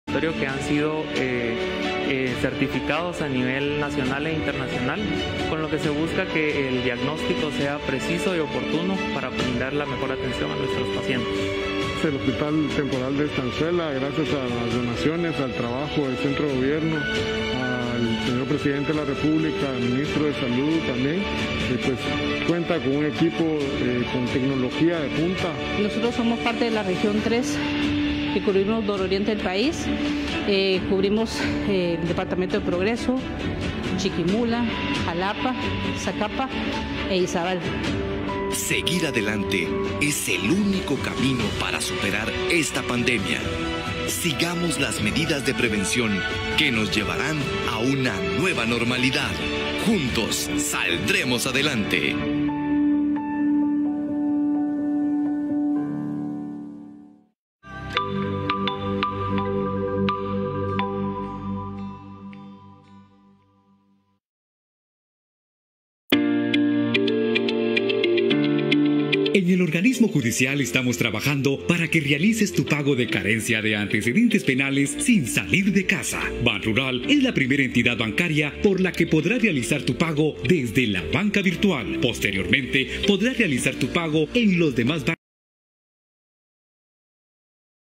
President Alejandro Giammattei on National Radio and Television Network from the Presidential House
President Alejandro Giammattei from Guatemala message has ending!